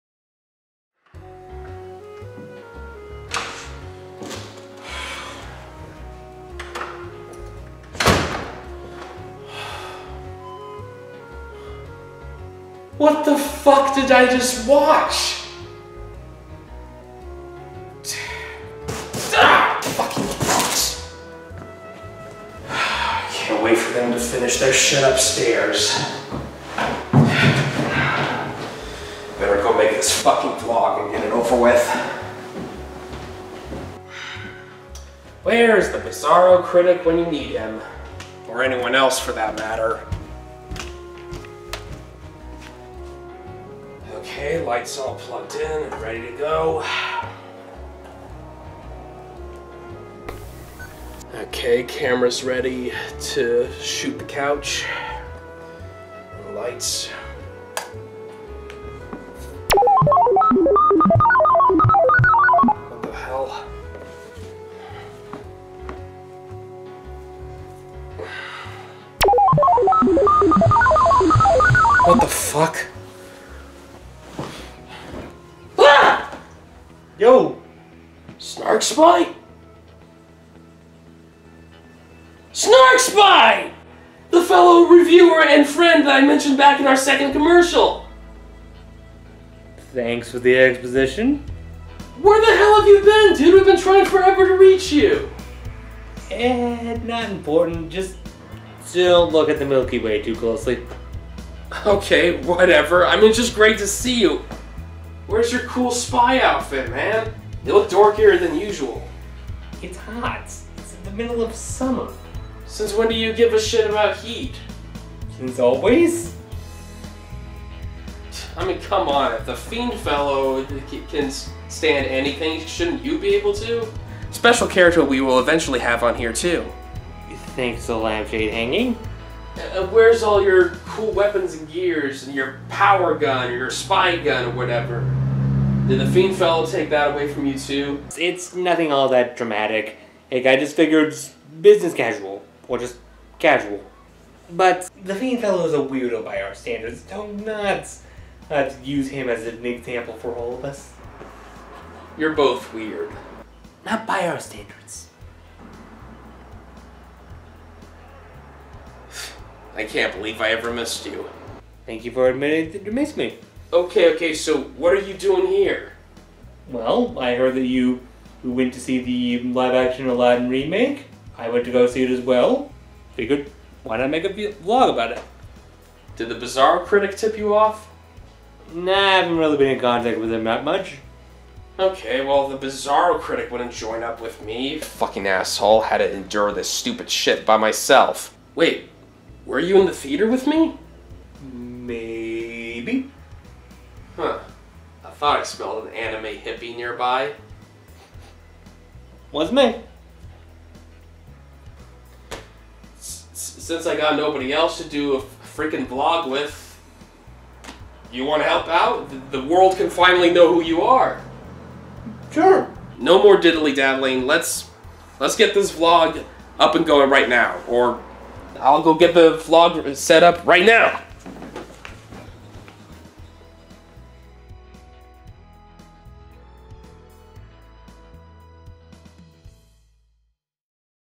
Snark Spy's First Appearance
TheWaltDisneyCompany Aladdin AmateurMasters SnarkSpy SpiritMan Check out this awesome Intro Scene Highlight from our Aladdin 2019 Vlog back in the day where the mysterious Snark Spy made his first epic appearance!